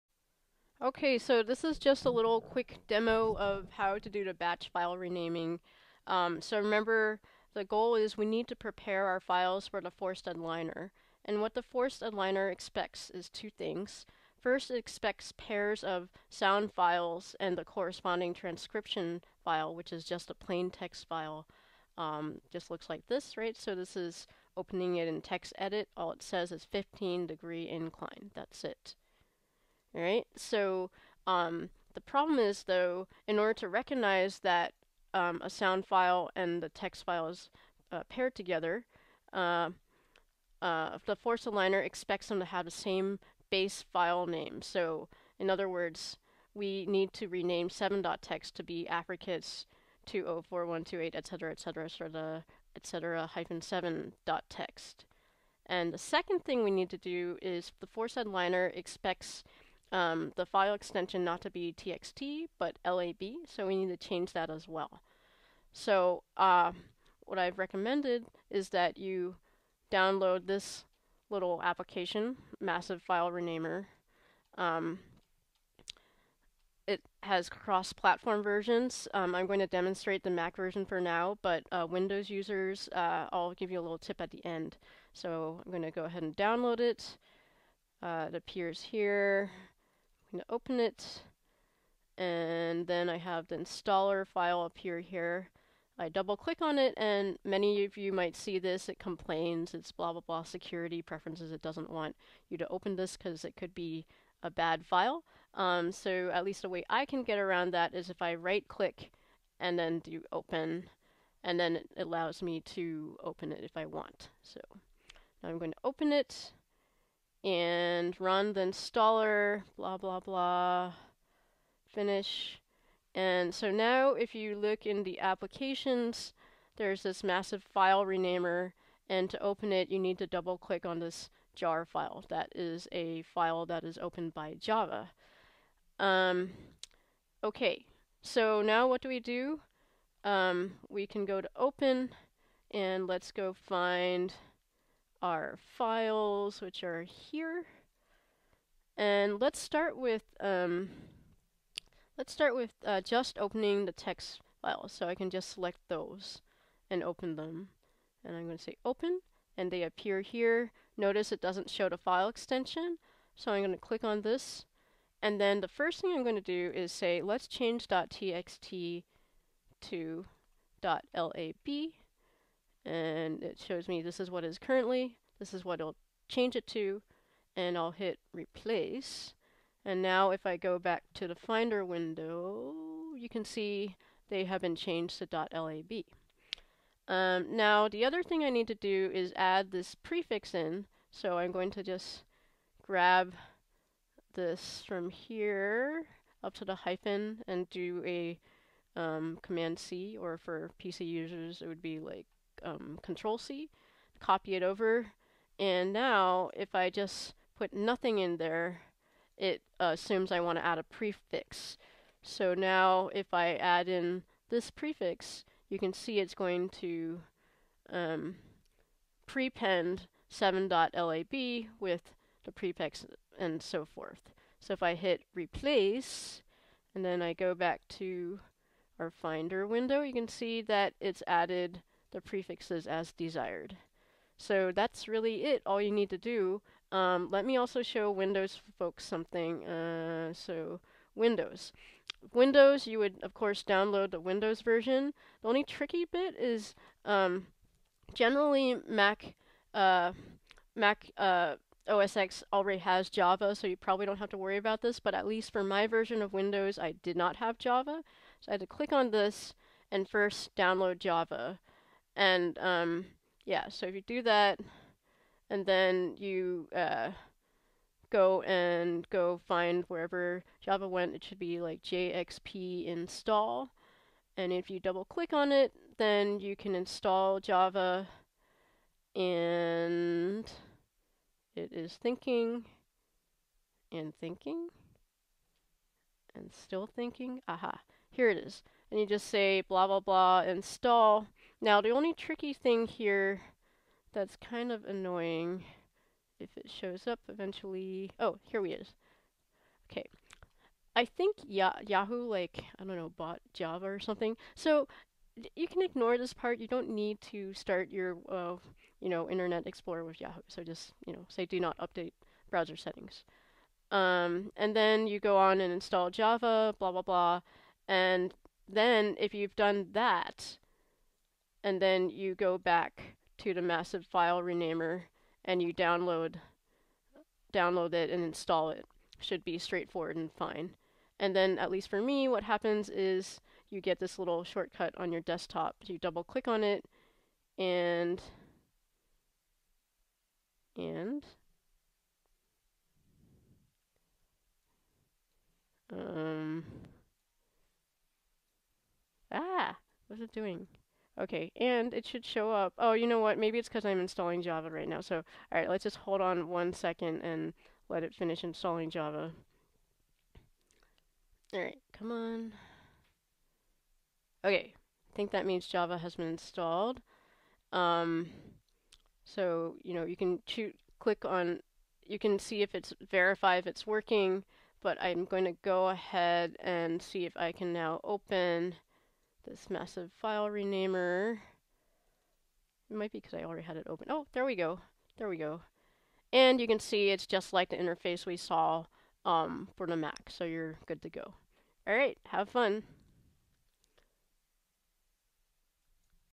Batch file renaming demo with Massive File Renamer
Demo of batch file renaming demo to prepare files for forced aligner, using Massive File Renamer.